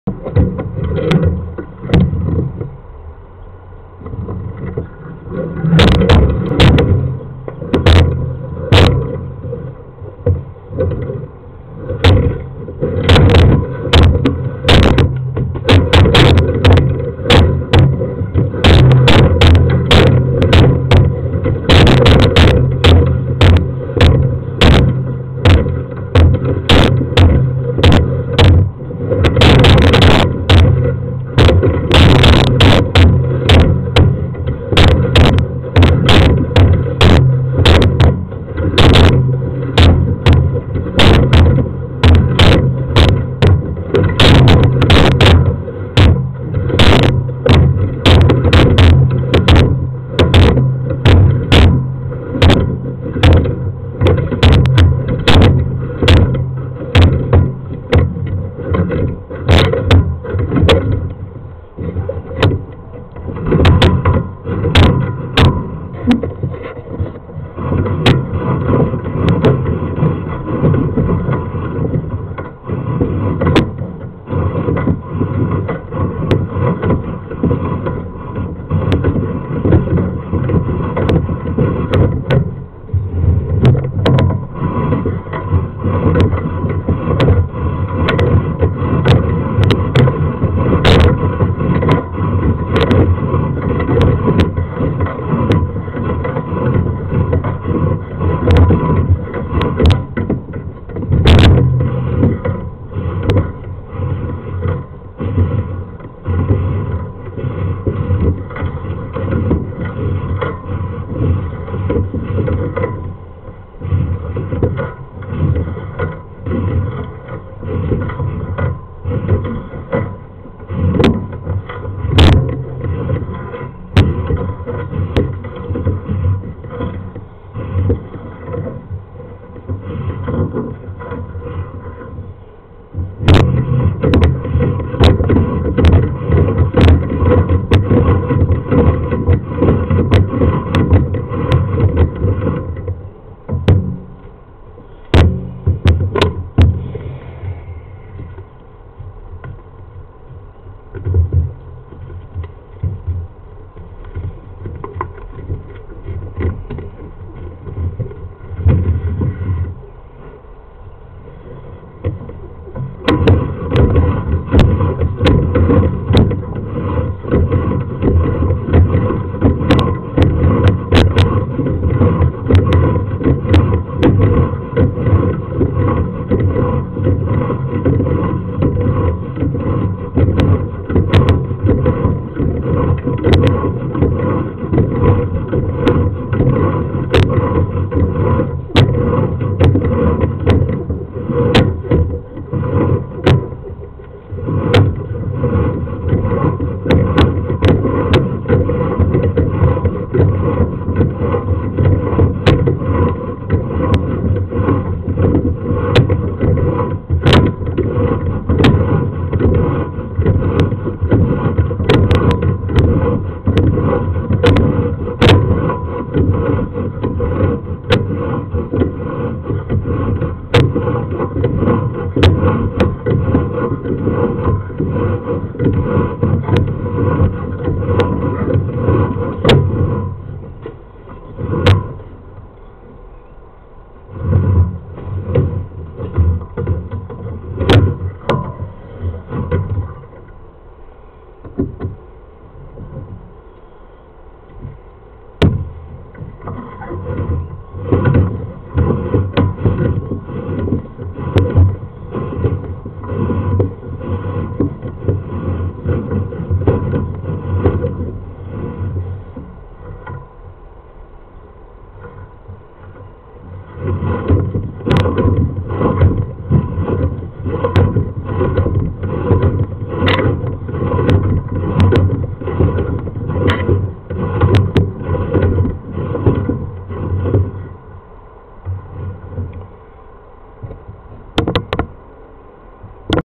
1917 proctor W Sac 10-7-21
Lateral line sewer inspection. No defects noted.